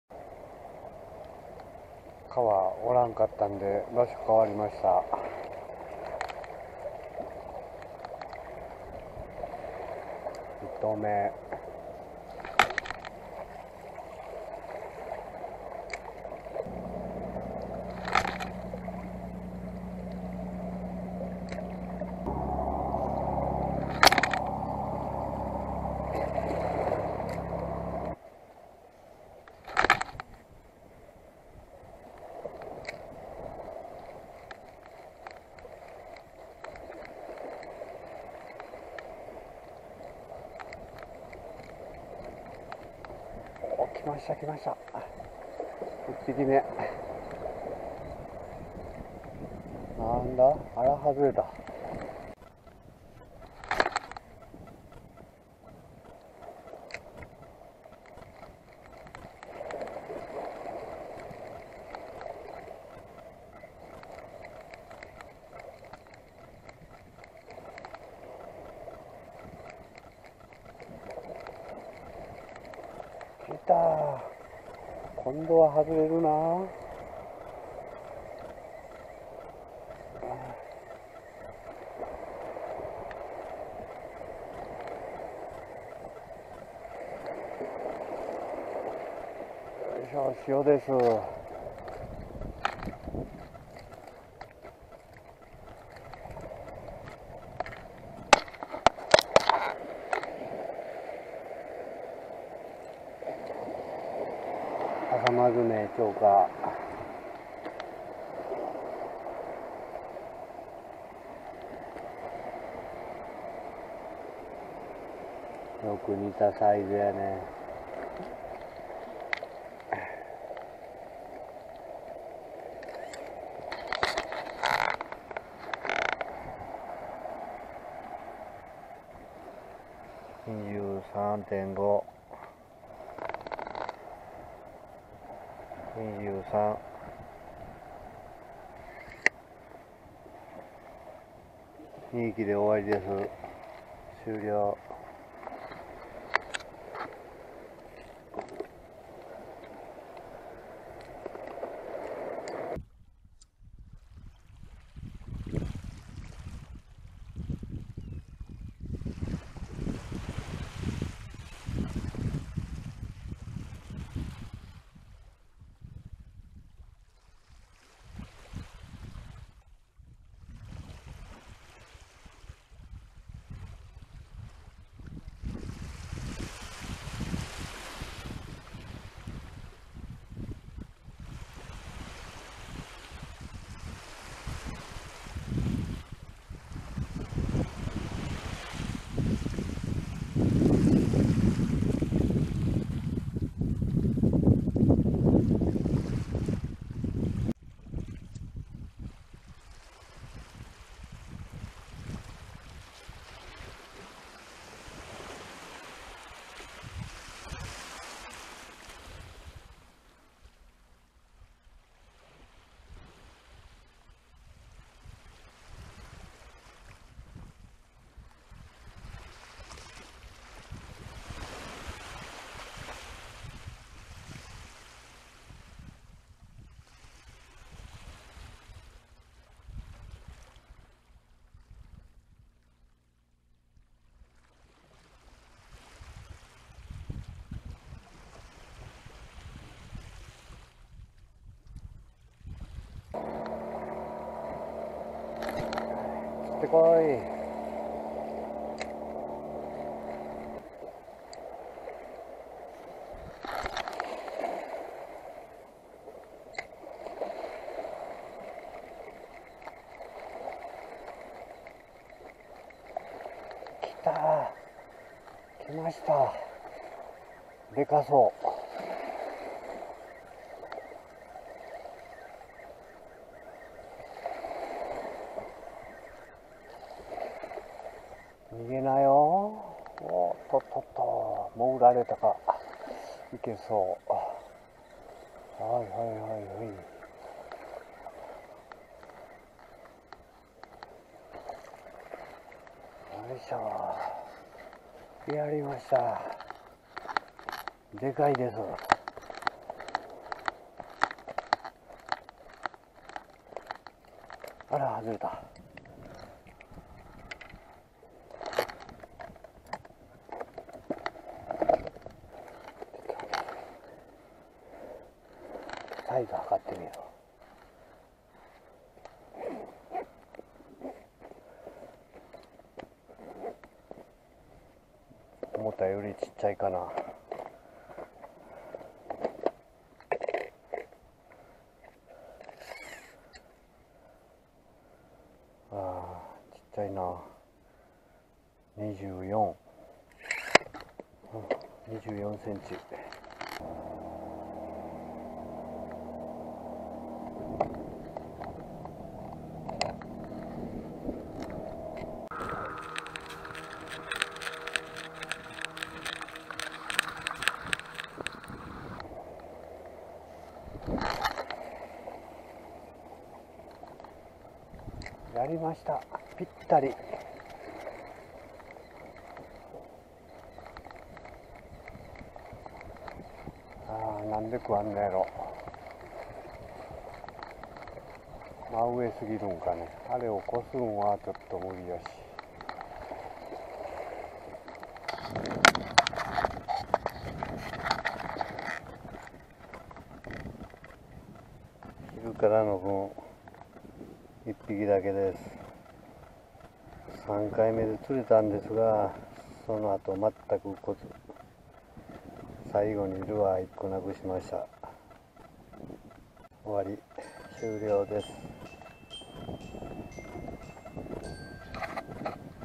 シオ狙い！魚はいなくなりそう・釣りカンパチ稚魚
シオ狙い釣りしました。
2022年7月1日撮影